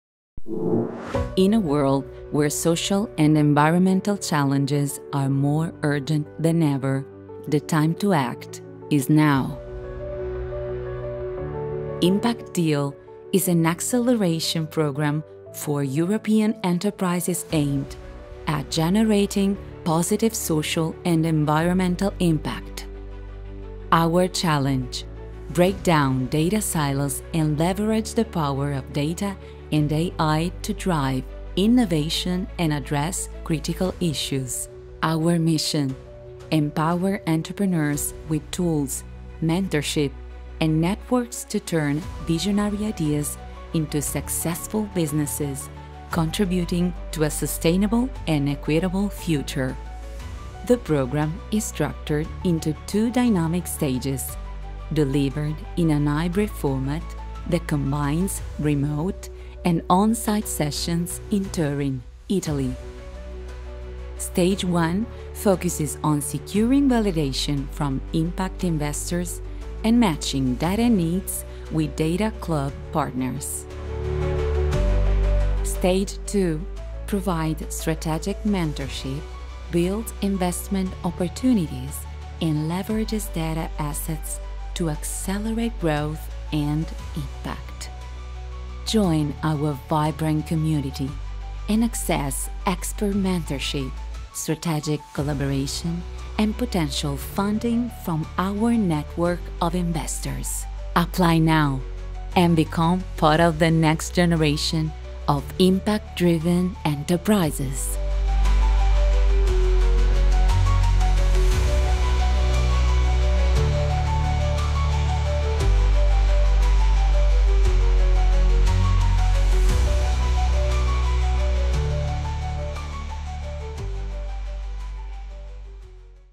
Impact Deal 2024 - TEASER - The time to act is now!
Impact Deal, now in its third edition, is a pioneering Data-driven Acceleration Program for European Impact Enterprises. This program provides essential resources — such as exclusive data access, mentorship, investment opportunities, and strategic partnerships — to elevate enterprises committed to achieving impact objectives aligned with the UN's Sustainable Development Goals (SDGs).

Impact Deal focuses on driving measurable positive impact on the environment and society, contributing to a sustainable and equitable future.

The program was founded and promoted by Fondazione CRT and OGR Torino, and it is managed by TOP-IX, with scientific supervision by ISI Foundation.